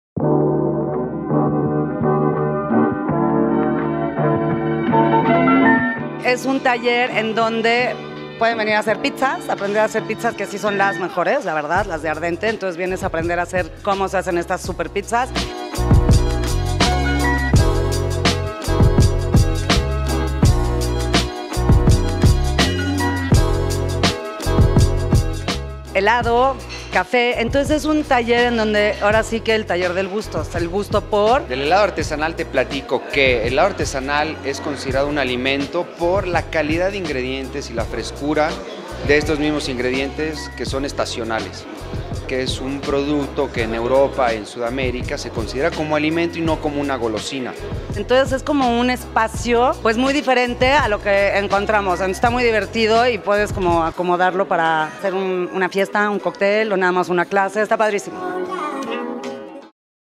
El Taller del Gusto te enseña a hacer pizzas, café y helados artesanales con tus amigos
¿Ya conoces el Taller del Gusto?

Es un espacio de enseñanza técnica y sensorial gastronómica

Entusiastas, amateurs y profesionistas del mundo de la gastronomía coinciden para mejorar su conocimiento y capacidad creadora. Josefina Santacruz, chef de Paprika y creadora también del Sesame, nos platicó que en este nuevo lugar podrás hacer pizzas, helados y tomar un delicioso café. Alfonso Jarero, maestro heladero y segundo lugar en el Sherbeth Festival, dice que “el helado artesanal en Europa y Sud América es considerado un alimento por su calidad de ingredientes estacionarios y frescura. Y no como una golosina”.